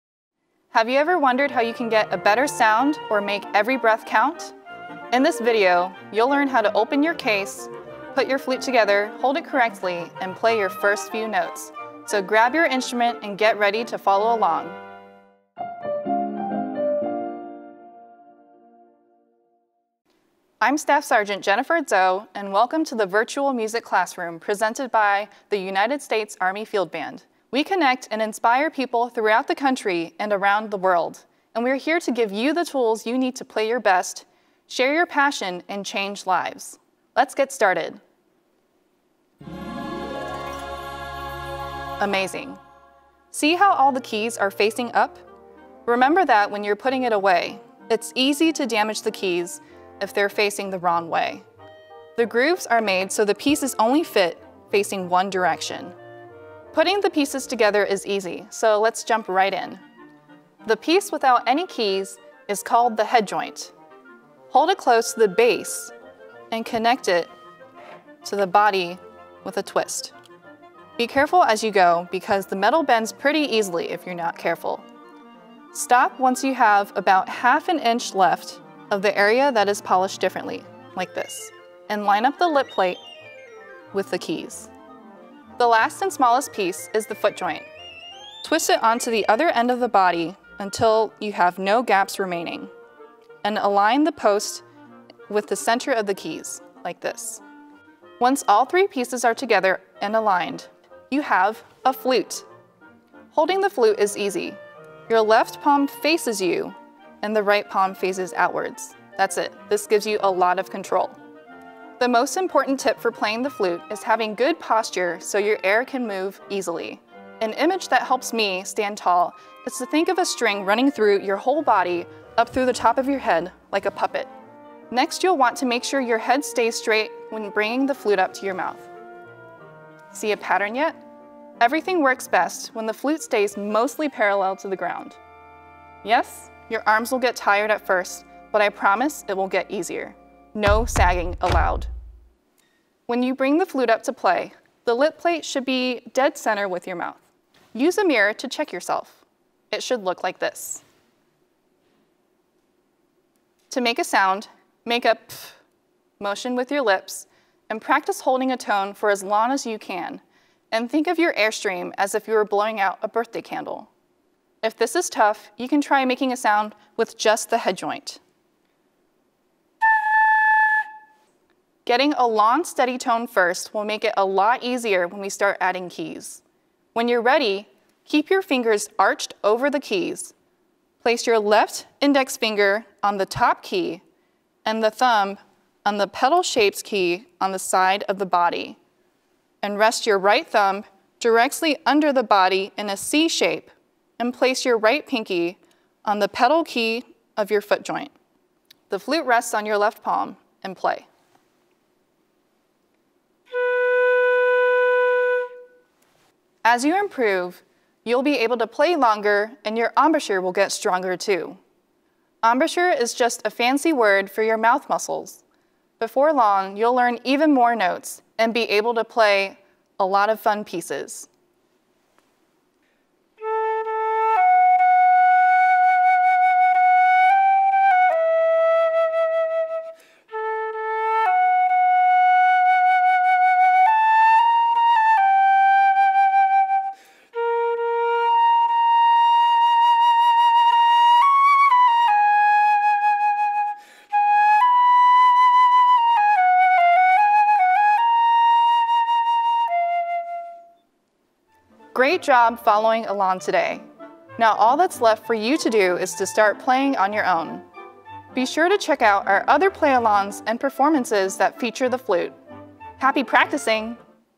Time for some flute! | Flute Basics with Staff Sgt. Jennifer Zhou
New to the flute?  Follow along with Staff Sgt. Jennifer Zhou gives us a crash course on getting started, the right way.  Grab your flute, and let's get started!

4214 Field Band Drive
STE 5330